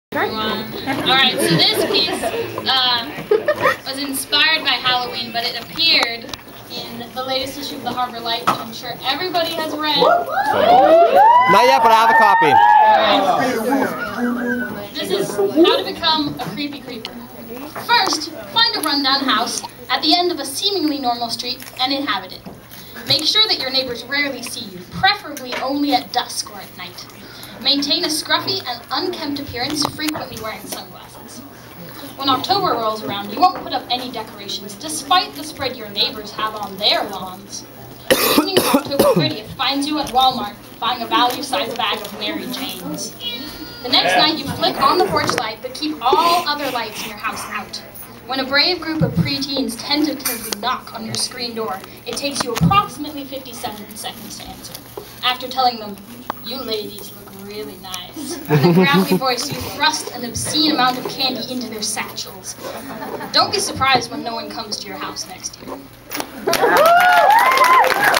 Phoebe- How to become a creepy creeper
Phoebe tells us how to become a creepy creeper.